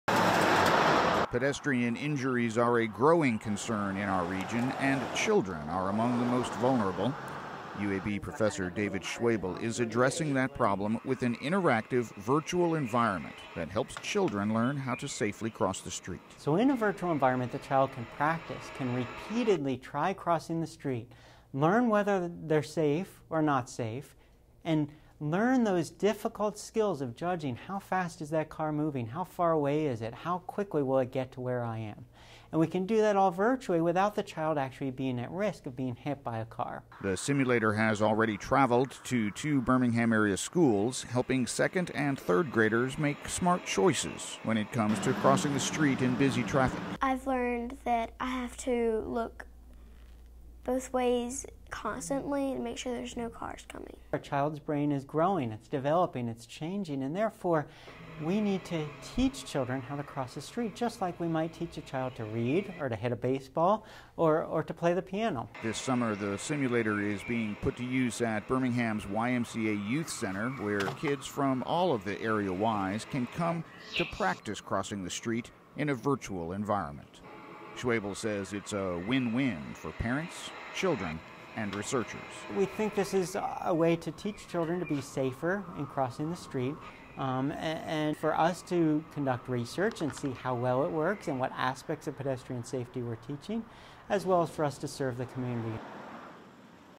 UAB Professor Combines Pedestrian Safety with Technology to Help Birmingham-area Children
A pedestrian simulator developed by University of Alabama at Birmingham professor David Schwebel is helping area children learn how to cross the street in a safe, yet realistic environment.